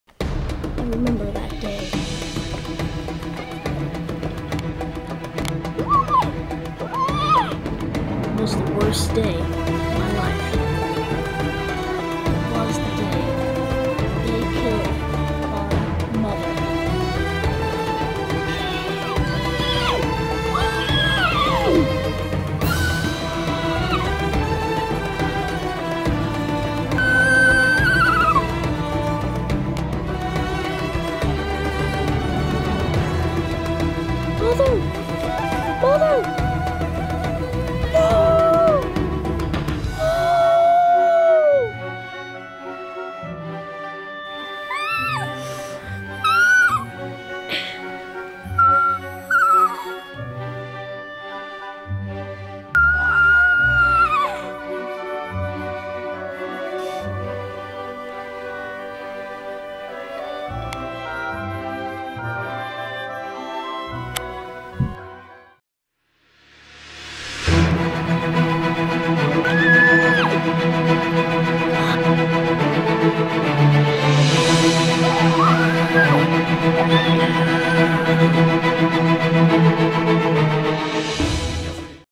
Breyer Horse Movie Trailer--Rescue Horse
This is my first Breyer horse movie trailer. I can't think of a name for the main horse character. Any suggestions? Hope you enjoy it! Please rate and comment! NOTE: The music does NOT belong to me.